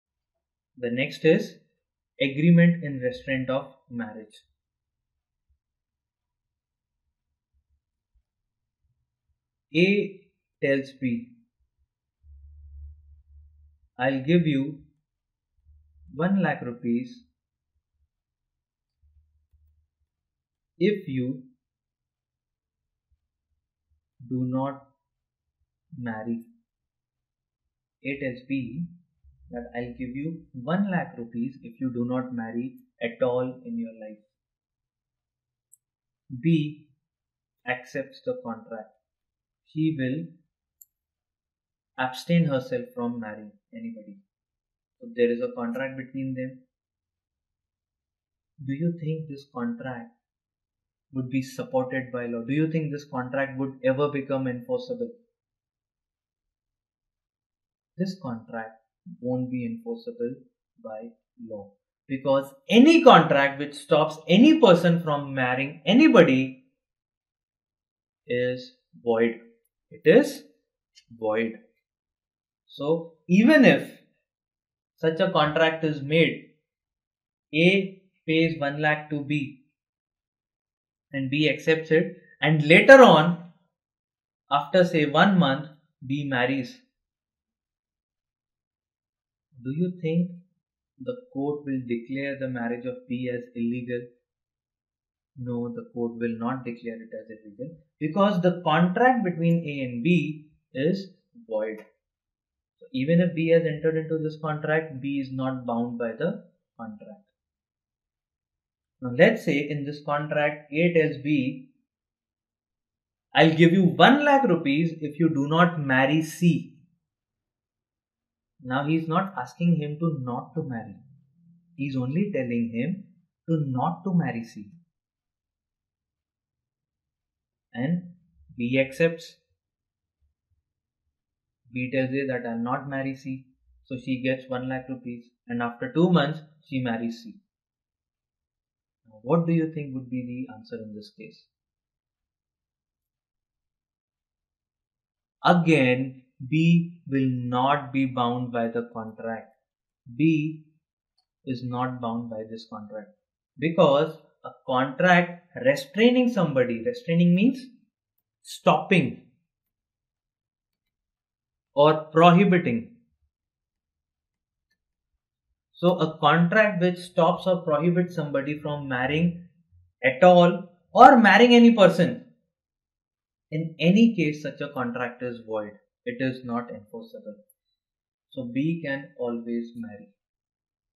Agreement in Restraint of marriage | Other Essential Elements of a Valid Contract | CA CPT | CS
Agreement in Restraint of marriage, Learn Other Essential Elements of a Valid Contract, Agreement Opposed to Public Policy, Capacity to contract, Free Consent and Agreement Opposed to Public Policy.